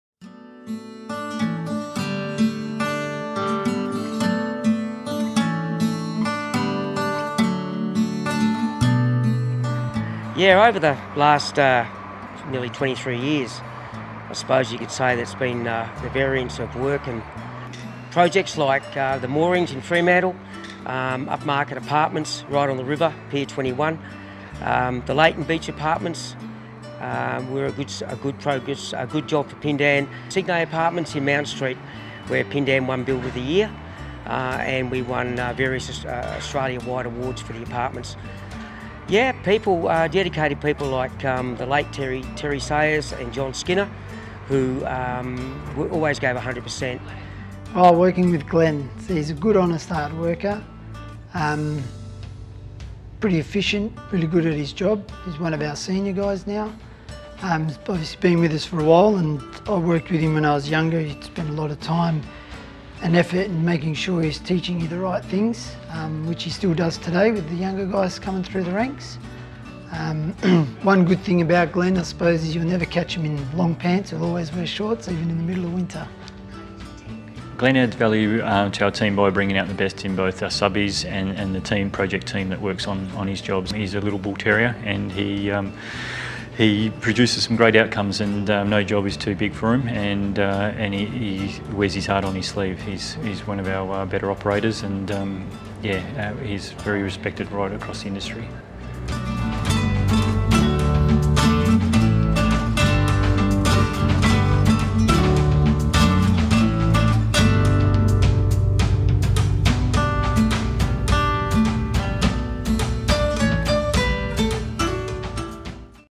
Congratulations Glen!
Congratulations on 20 Years at Pindan Glen! 🎉

Hear what Glen and some of his colleagues have to say about his time at Pindan so far.